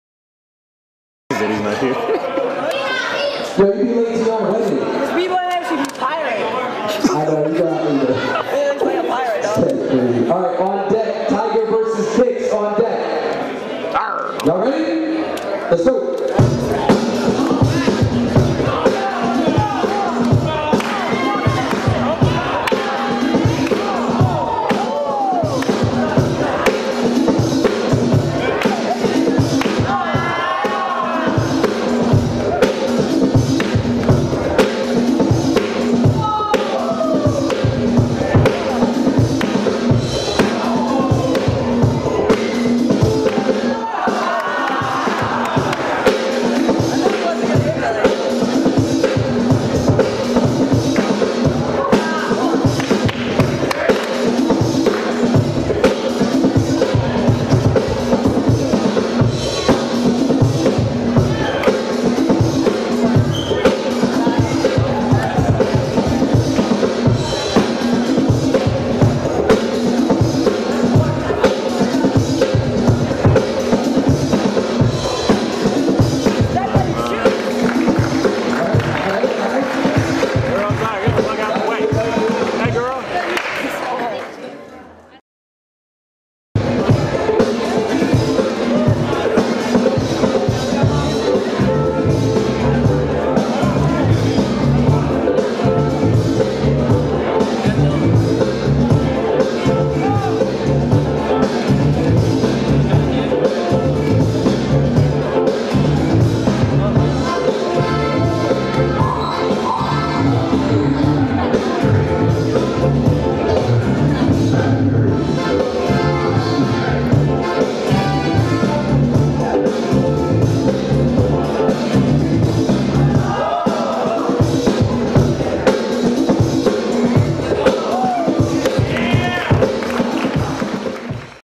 2010 S.E.B.C. 1 vs 1 BBoy Battle | Top 16 | Illness vs Panik
Southeast Bboy Championship 1 vs 1 Bboy Battle for $600 and the crown for best bboy in the southeast.